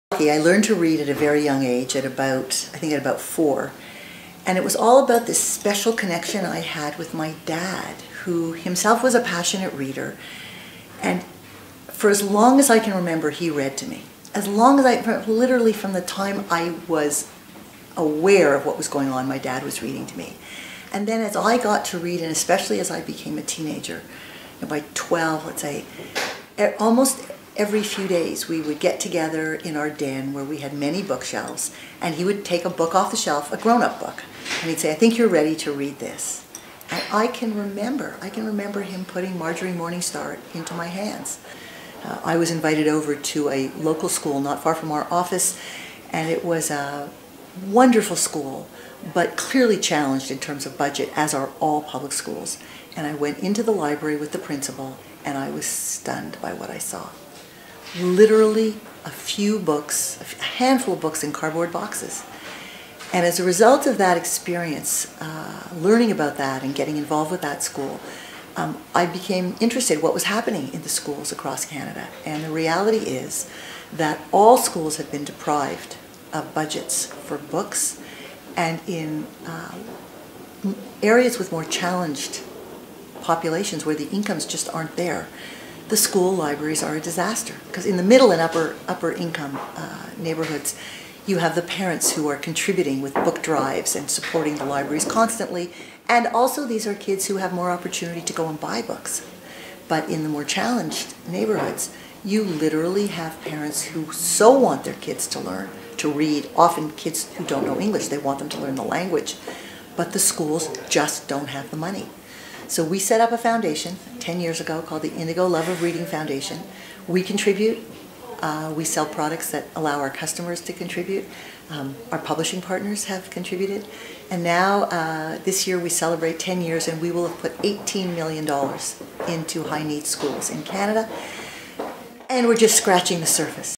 Indigo Chapters' Heather Reisman
Heather Reisman, CEO of Indigo, talks about how important reading is in the development of young children.